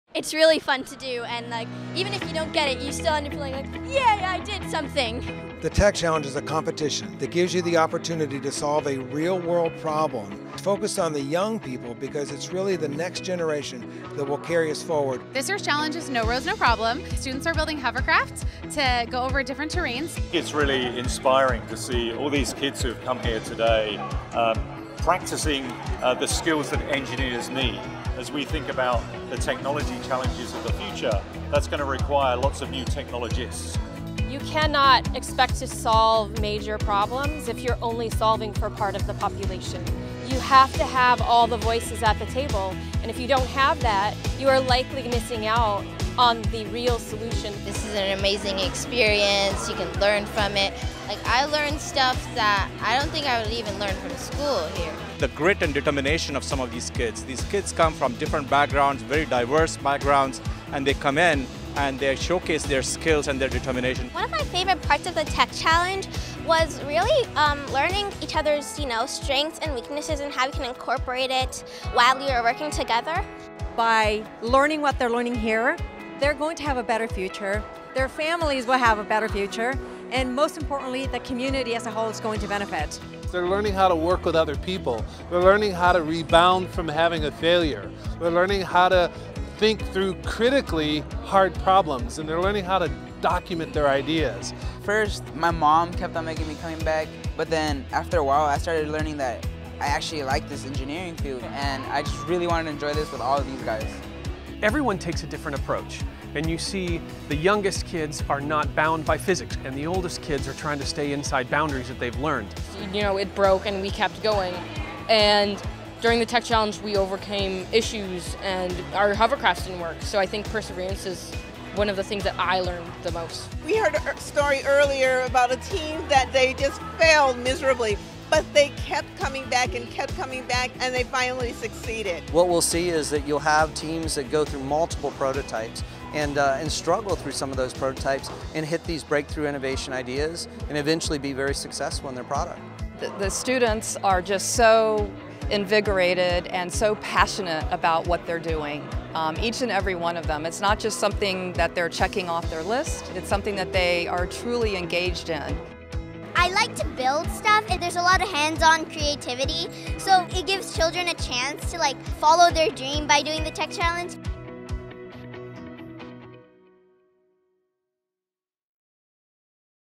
The Tech Challenge 2019: No Roads. No Problem!
Thousands of students from across Northern California and beyond participate in the 32nd annual Tech Challenge, presented by Dell.

The Tech Challenge is a signature program of The Tech Interactive that invites teams of students in grades 4–12 to use engineering design to solve a real-world problem. It reinforces 21st century skills including creativity, problem solving, design, teamwork, leadership, risk-taking, perseverance and learning from failure.

The 2019 challenge asked teams to build a hovercraft to cross different terrain. Students traveled from as far away as Canada and Colorado to participate in the event March 30 and 31.